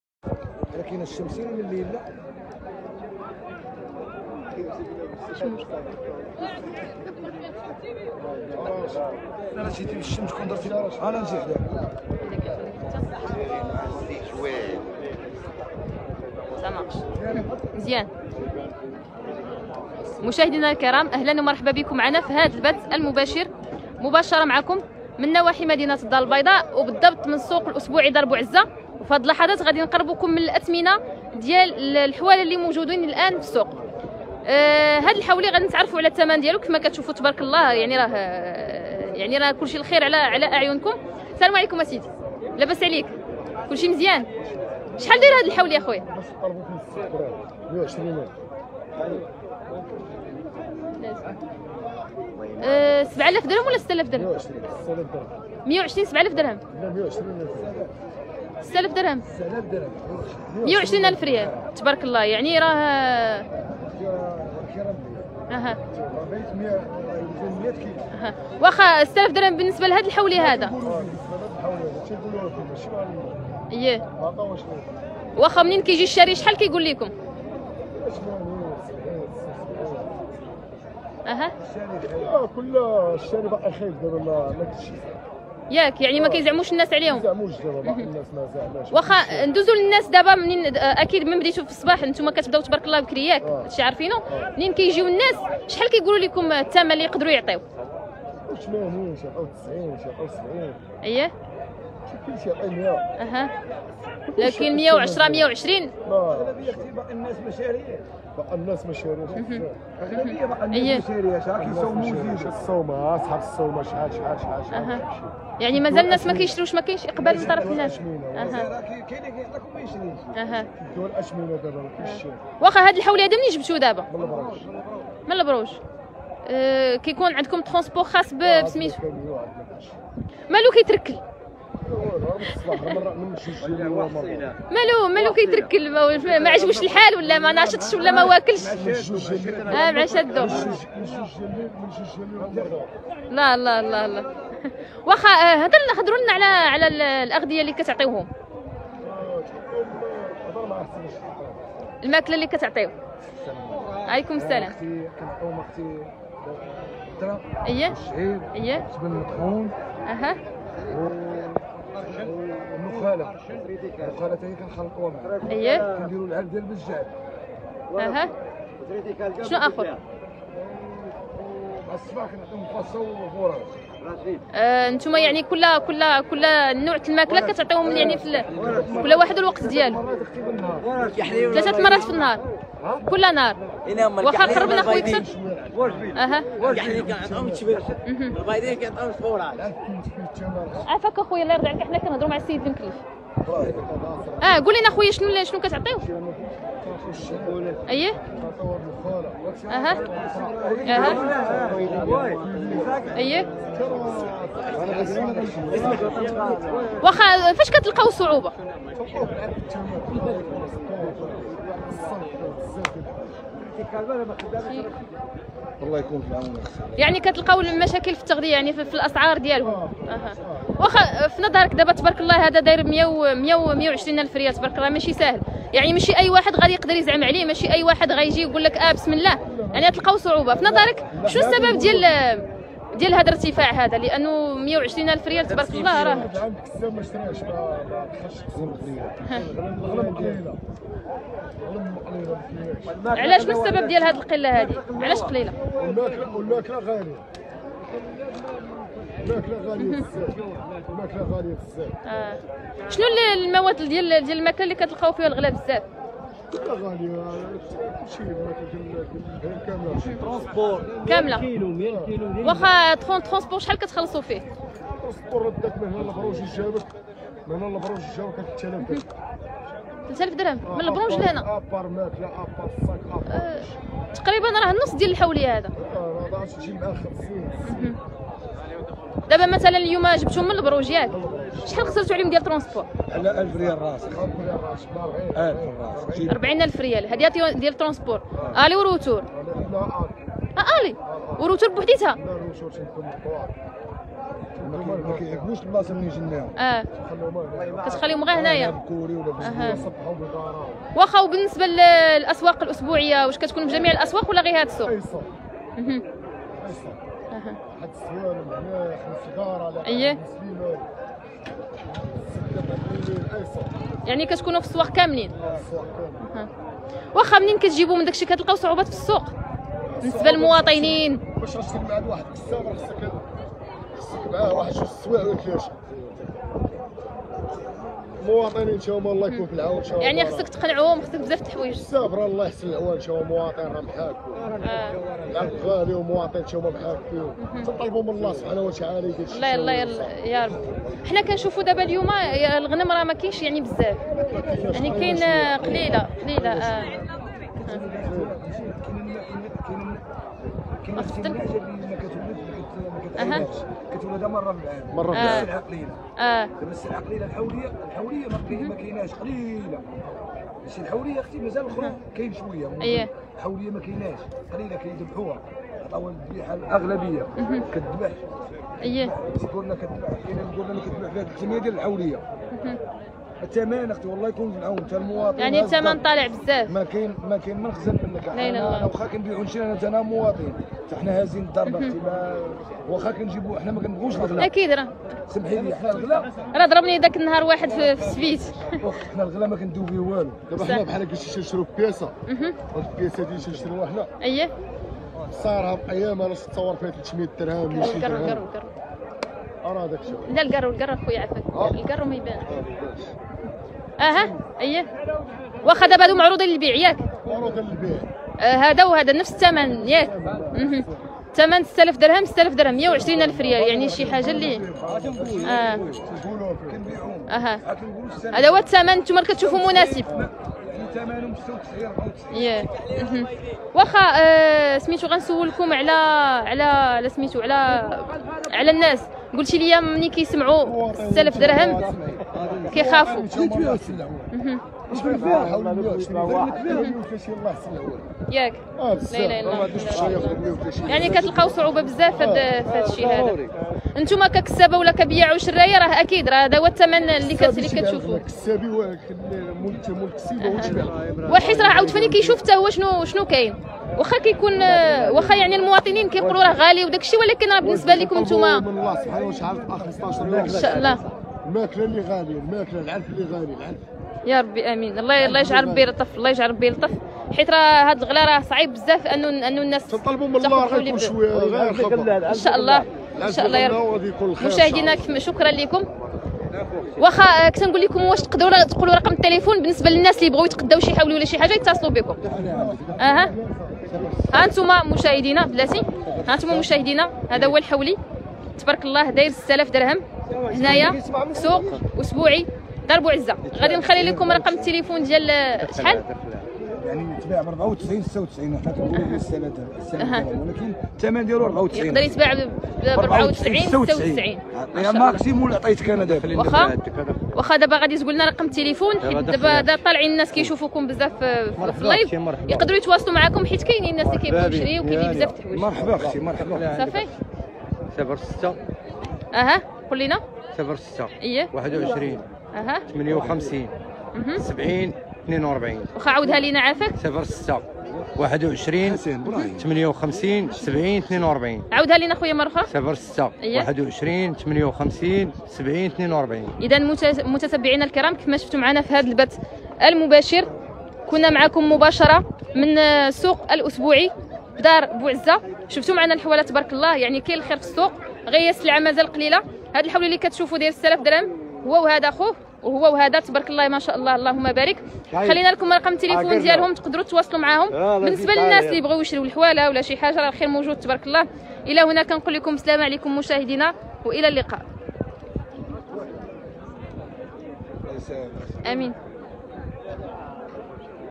أكبر حولي ساوي 6 آلاف درهم عينيه كحلين وسنان الحليب والشكل ديالو زوين بزاف فالسوق الأسبوعي دار بوعزة
عاجل:أكبر حولي ساوي 6 آلاف درهم عينيه كحلين وسنان الحليب والشكل ديالو زوين بزاف فالسوق الأسبوعي دار بوعزة